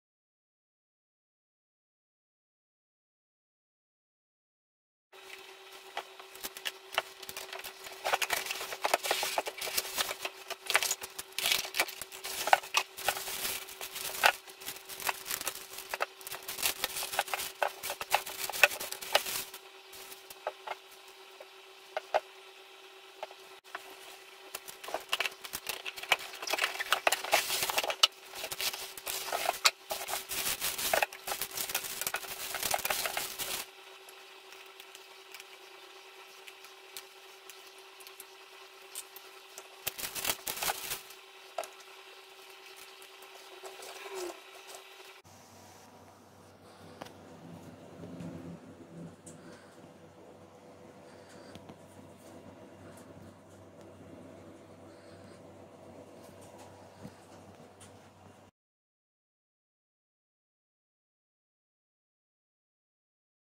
Unboxing One Piece BWFC Banpresto World Figure Colosseum 10th series (Luffy gear 4 and Ace Figurine)
Unboxing Luffy Gear 4 and Ace from BWFC 10th series.